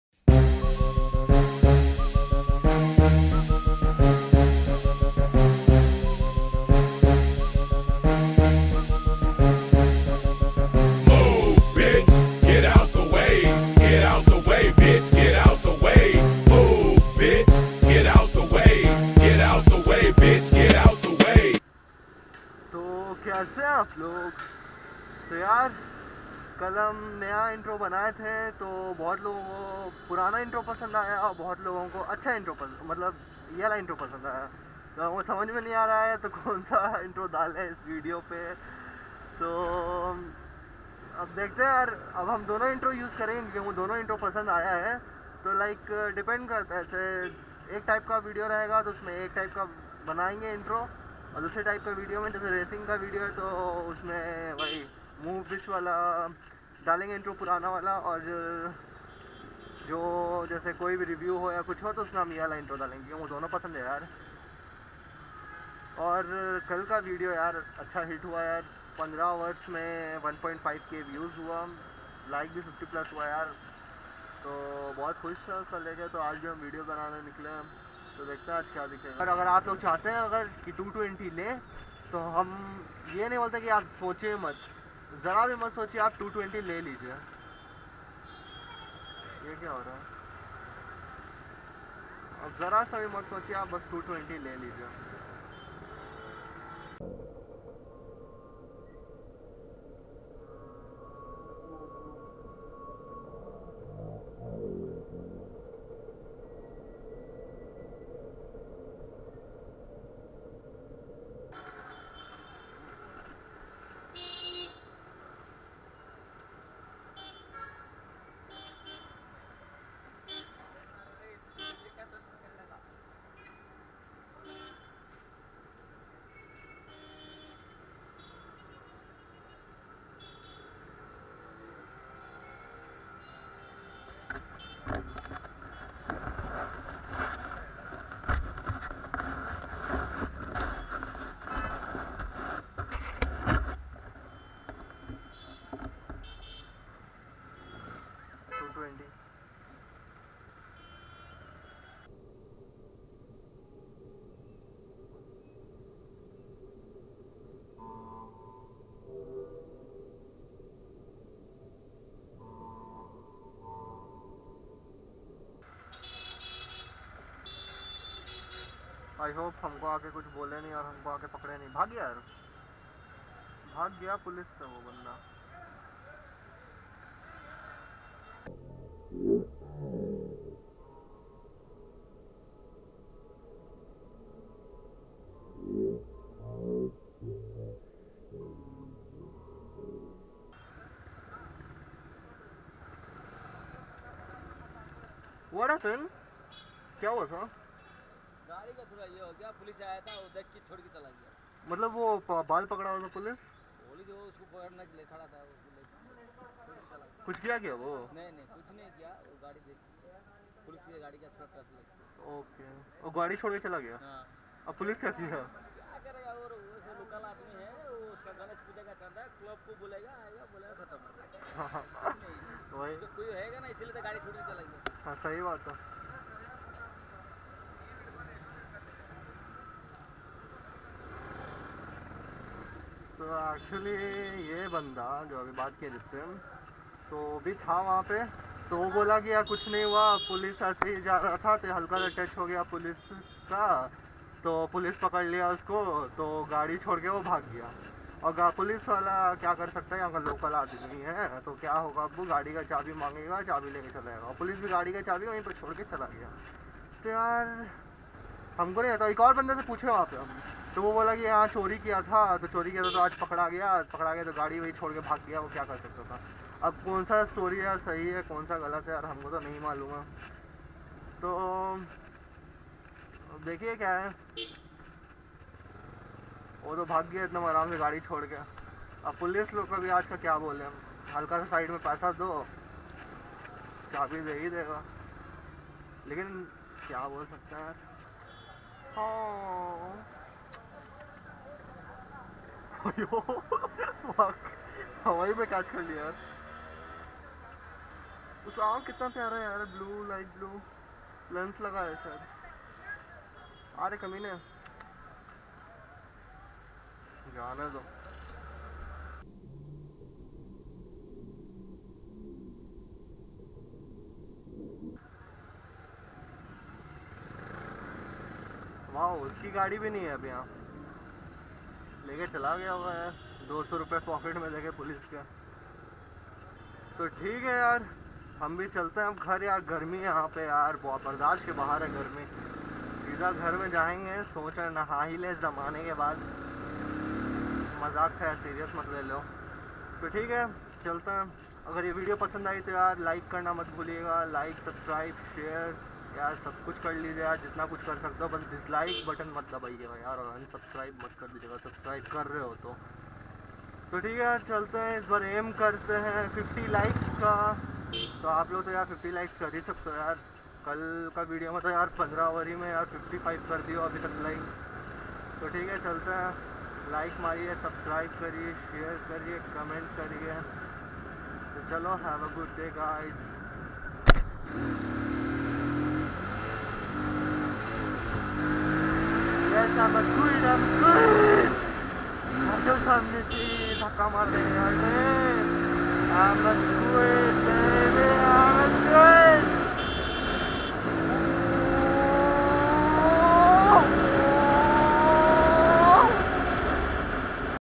Police beating a guy in public | Rage | Must watch
today while recording video i saw a police beating a guy. must watch